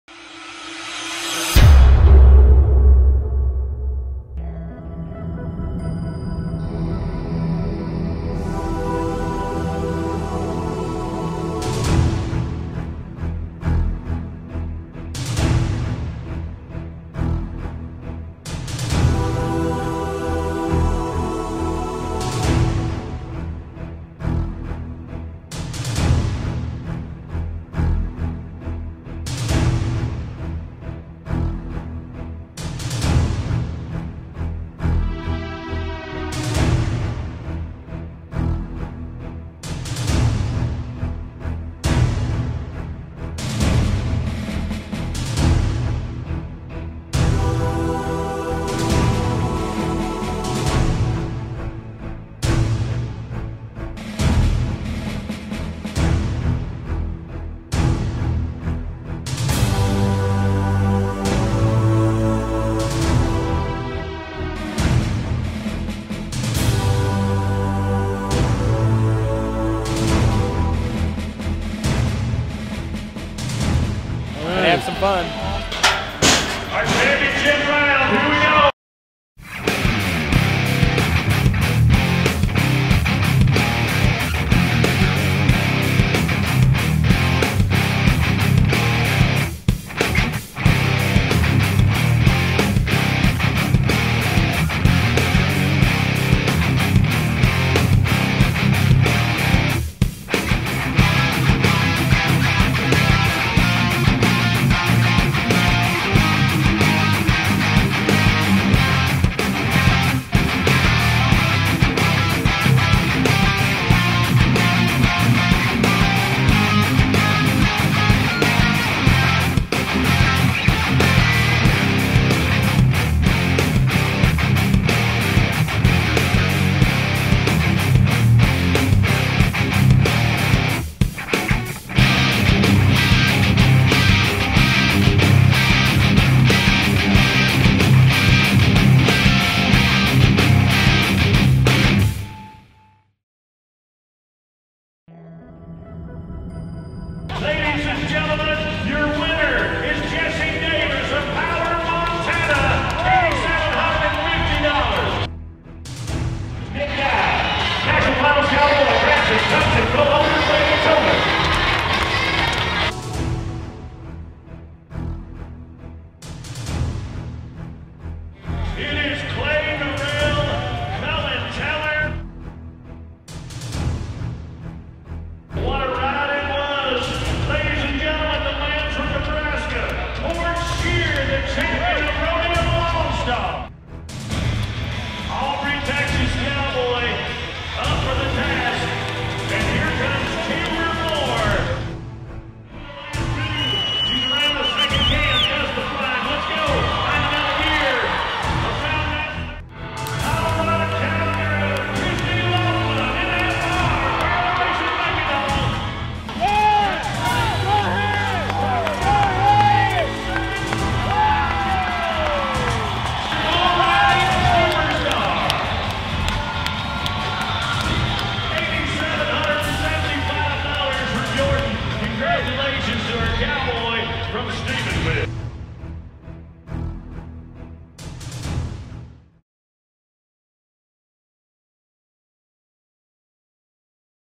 Rodeo All-Star 2014 Champions
Congratulations to the 2014 Rodeo All-Star Champions. Bareback - Jessy Davis, Steer Wrestling - Nick Guy, Team Roping - Clay Norell & Cullen Teller, Saddle Bronc - Cort Scheer, Tie-Down - Timber Moore, Barrel Racing - Christy Loflin and PBR Bull Riding - Jordan Hupp